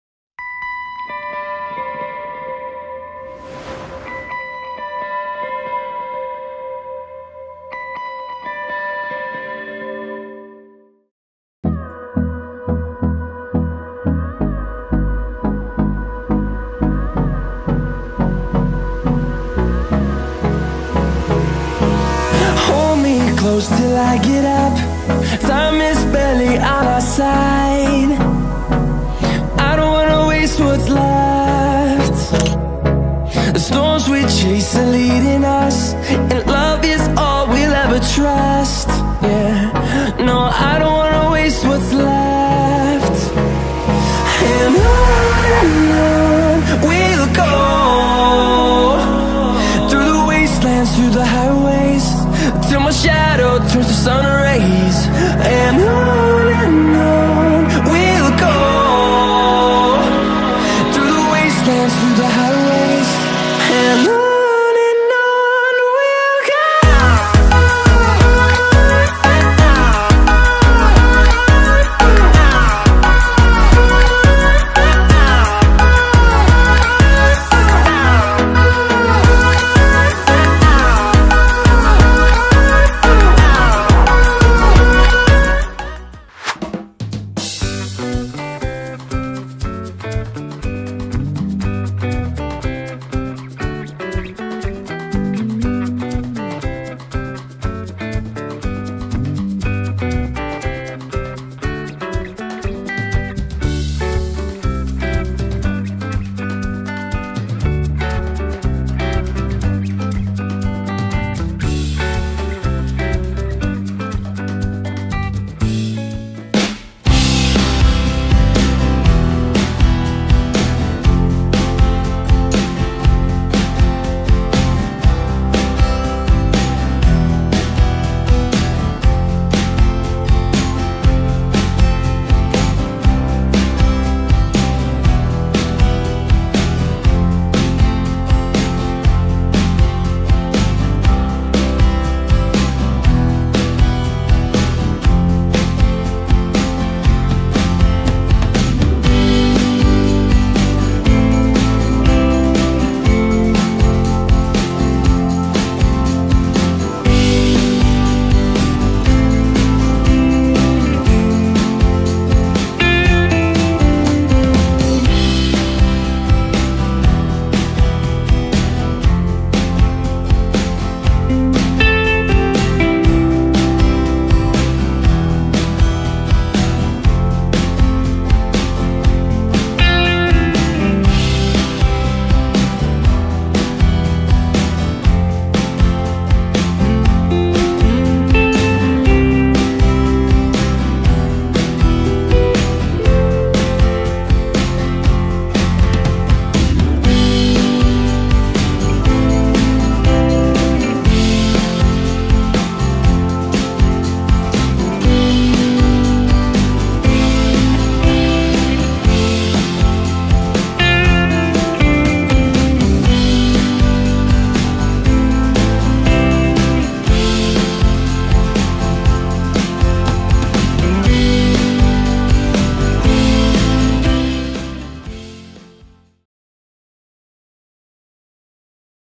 Equinox | Annual Science Expo | The Krisar Academy
Equinox's journey was a remarkable success! krisarites proudly presented 17 Sustainable Development Goals, captivating us all with stunning models and empowering speeches. Together, we embraced a grand vision for a better world. Let's continue to inspire change and make a lasting impact!